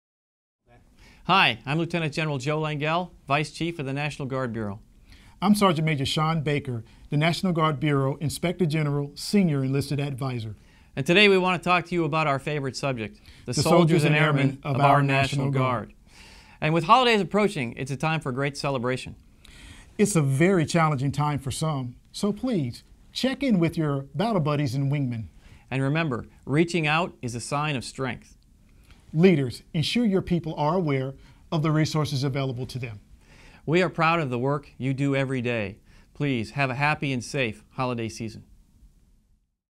Lt. Gen. Lengyel and SGM Baker share an important message
The holidays are a time of great celebration that also can be challenging for some. Take time to check in with your Battle Buddies and Wingmen. Lt. Gen. Joseph Lengyel, Vice Chief National Guard Bureau and Sgt. Major Sean Baker, Senior Enlisted Advisor, NGB Inspector General, wish you, your families and friends a safe and happy holiday season.